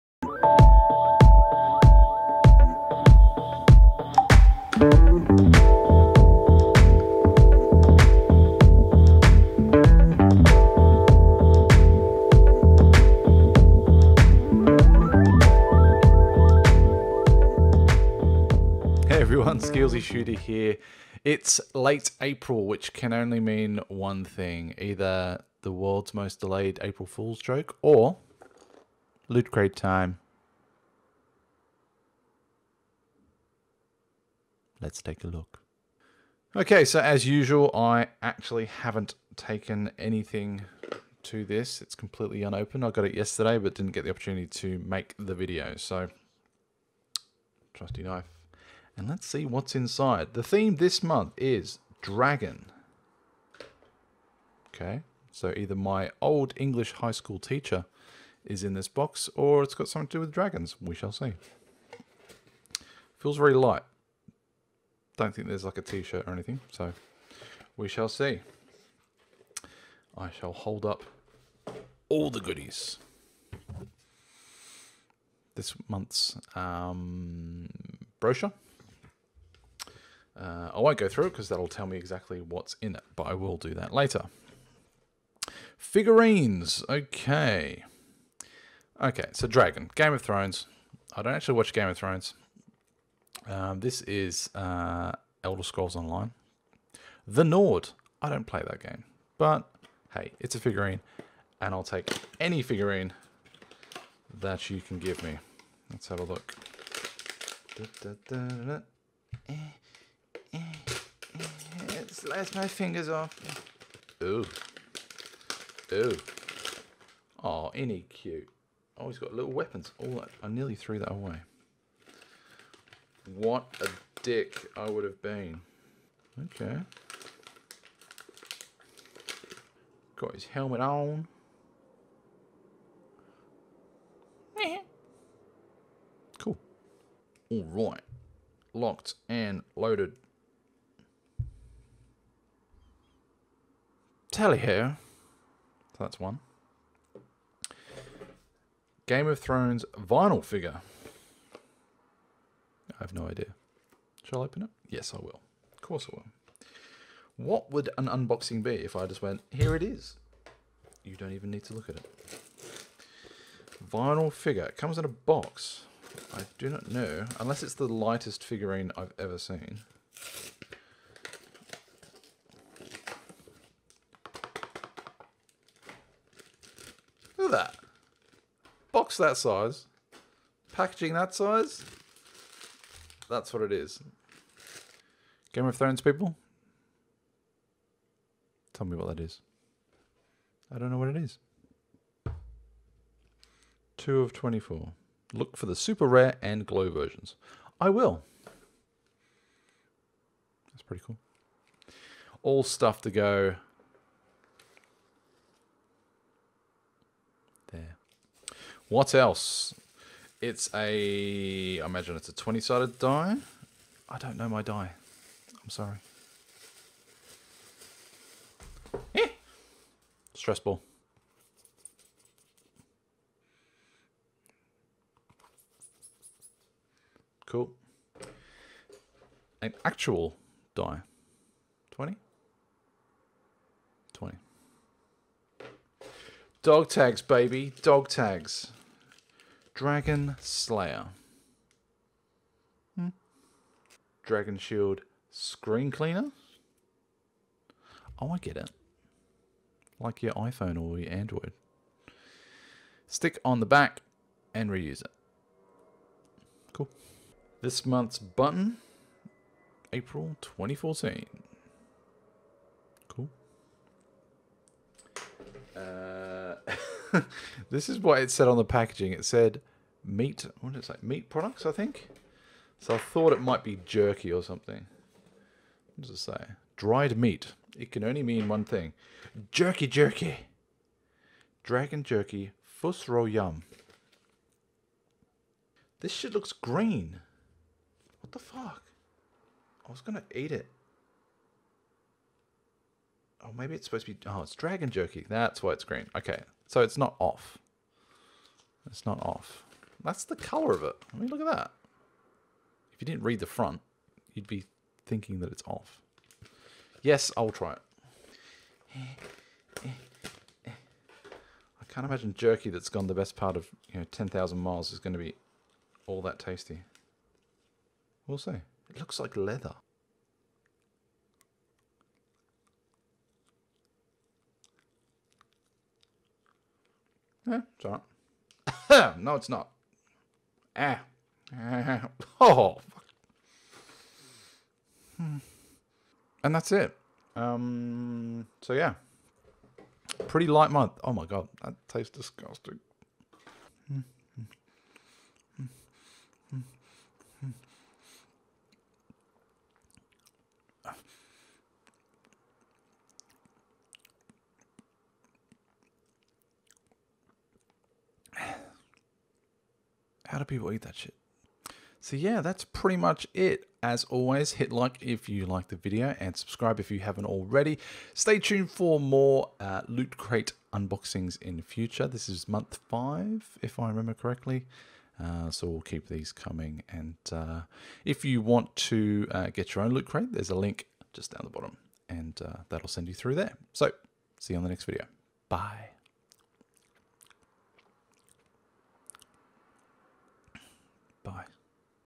Loot Crate unboxing April 2014 (Dragon)
The world's most sought after black box. well next to the MH370 one is back for April.

Featuring some cool figurines and some ordinary dragon jerky (anus) don't miss out and click on the below link if you want to sign up for your own LootCrate :)